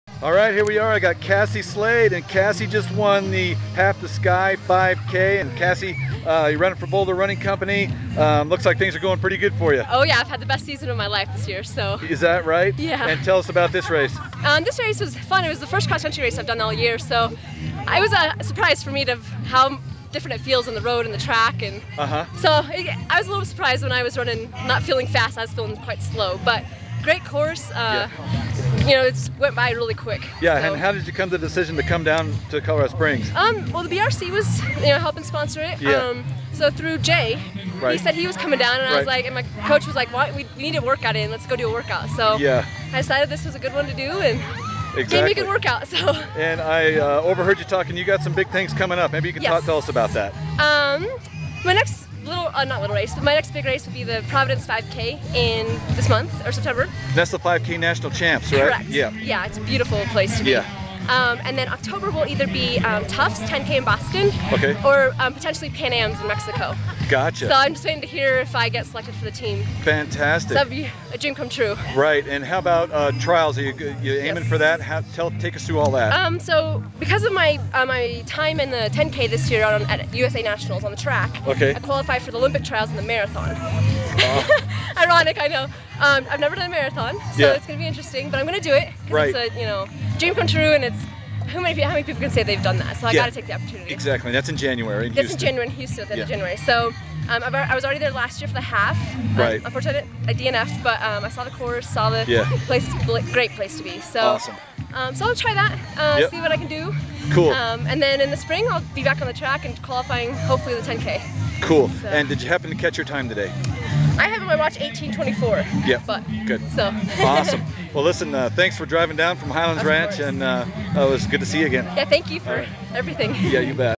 Interview with Cassie Slade, Half the Sky 5K women's race winner
Cassie Slade, 24, of Highlands Ranch won the Half the Sky 5K on Saturday, Aug. 27 at Doherty High School in Colorado Springs. Slade, who is training for the U.S. Olympic Marathon Trials in January in Houston, finished fourth overall with a time of 18 minutes, 24 seconds.